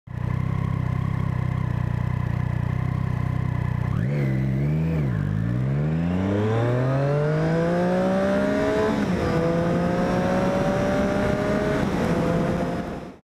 TRAILER: Essai Triumph street triple s 2017 A2
hey les amis ;)

un petit trailer pour la vidéos qui va sortir le 11 ou le 12 novembre.

Ride safe V